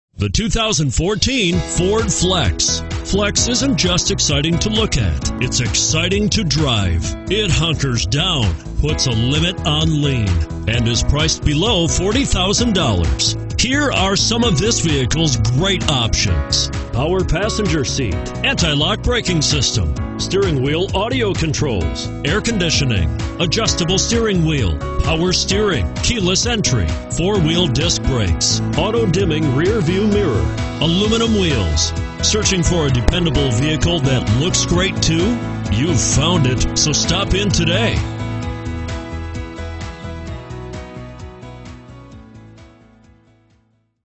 2014 FORD FLEX Taylor, MI 15436
2014 FORD FLEX Taylor, MI
Stock# 15436

Taylor Ford
10725 South Telegraph

This red 2014 Ford Flex has everything you need. This one's a deal at $37,560. Heated seats keep everyone comfortable. Even the passengers in the back seat will ride in comfort thanks to the rear air control. Reverse this vehicle with the utmost confidence using the backup camera. Equipped with a power driver's seat, this vehicle guarantees comfort from beginning to end. Keep your belongings safe and secure with the cargo net. According to a review from CarsDirect, Inside and out, the Flex is dazzling. Panel gaps, paint, surface textures-all are superb.